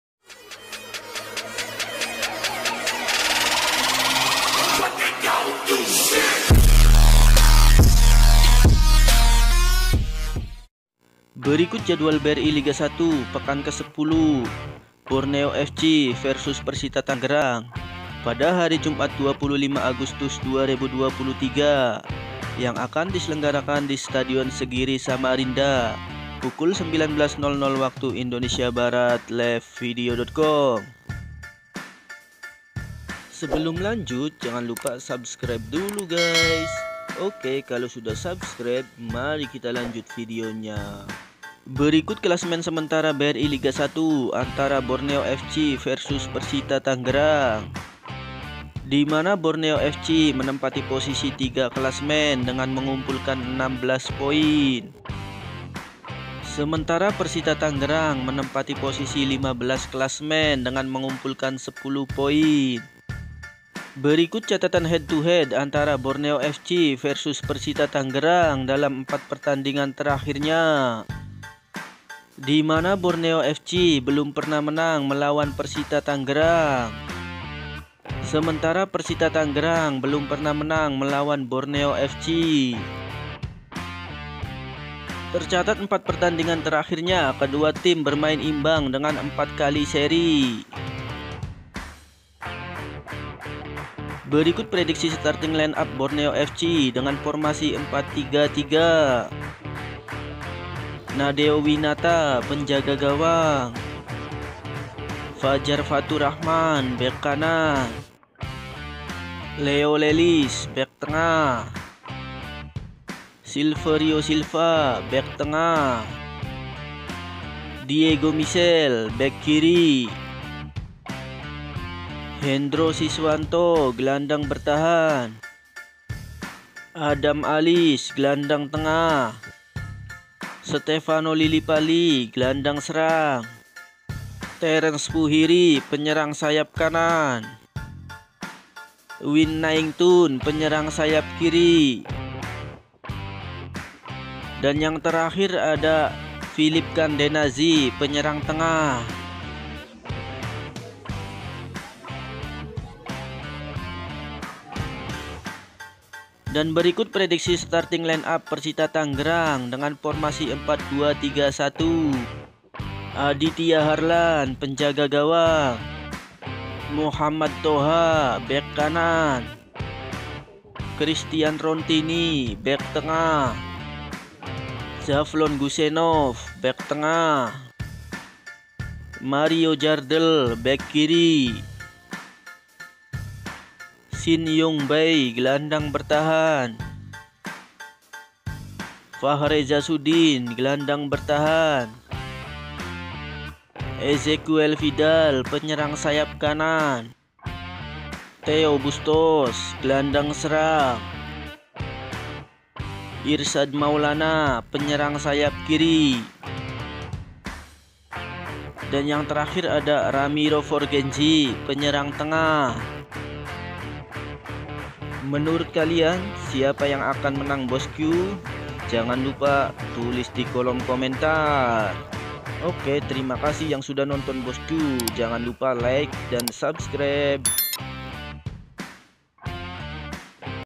BORNEO FC VS PERSITA ~ Prediksi Starting Line-up BRI Liga 1 2023 Pekan 10 ~ Live Video
Berikut jadwal bri Liga 1 pekan ke 10

Borneo fc vs Persita Tangerang
Jumat 25 Agustus 2023
Std. Segiri Samarinda
Pkl 19:00 Live  Video com